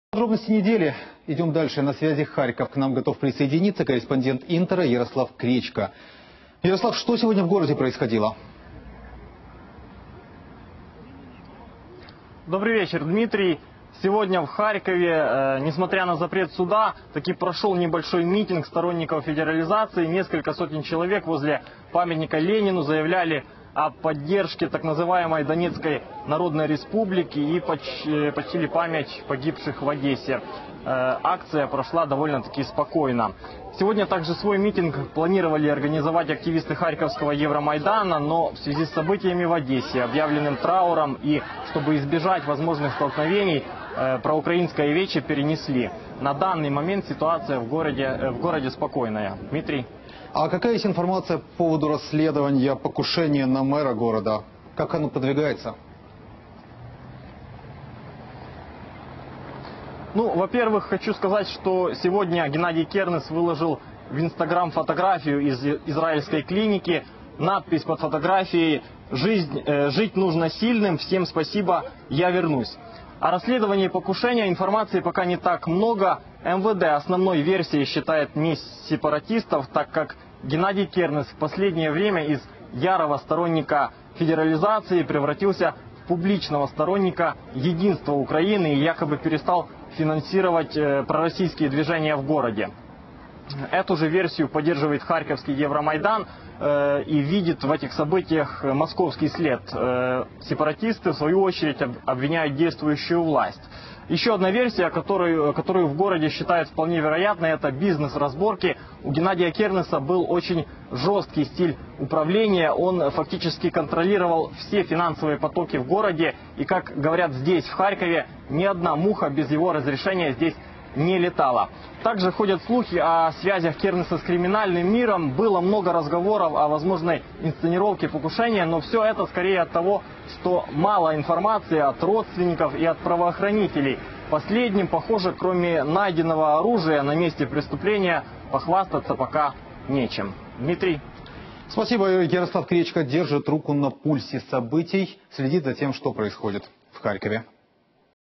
В Харькове митинговали сторонники федерализации
В Харькове митинговали сторонники федерализации -  - Интер - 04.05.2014

In Kharkiv rallied supporters of federalization - and inter - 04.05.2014